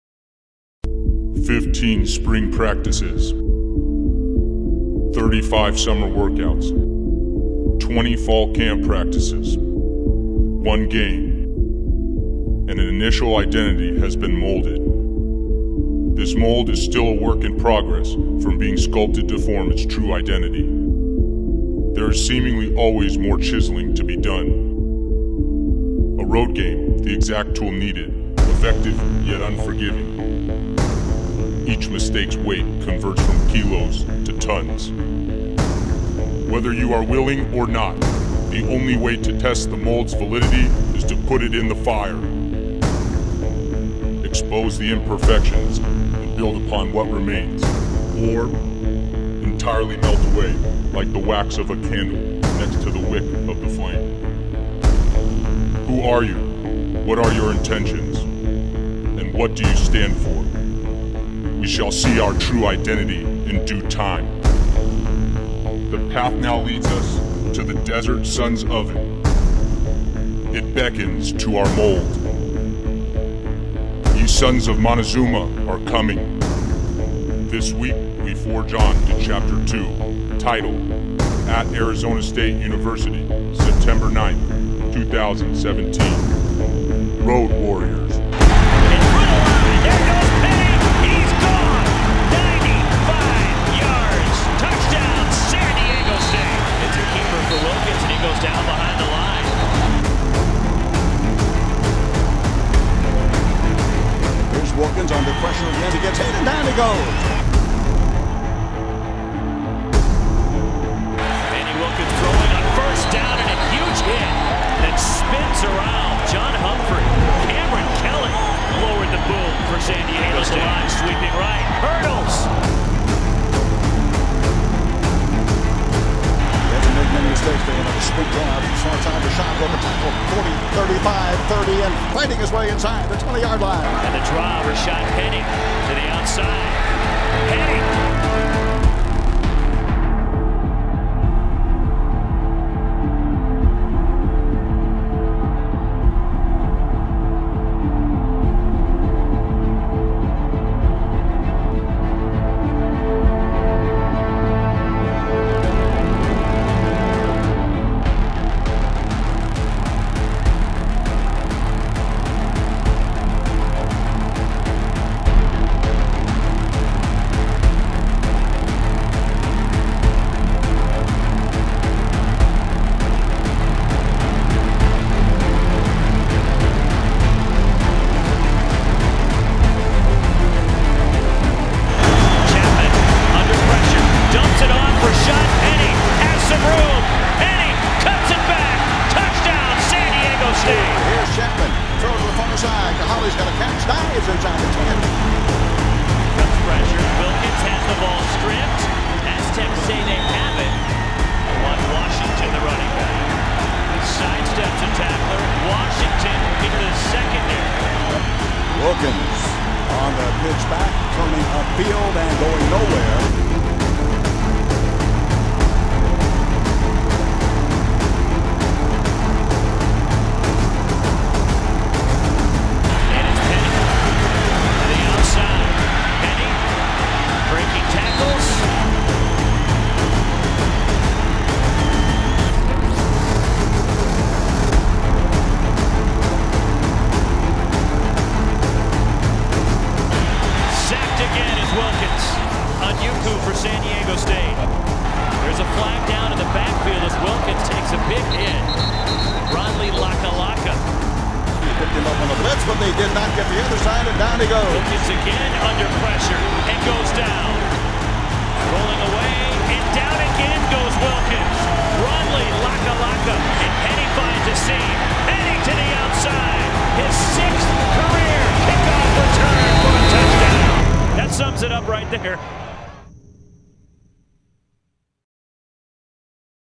2017 San DIego State Football vs Arizona State Highlights
2017 San DIego State Football vs Arizona State played on September 9, 2017 in Tempe, Arizona.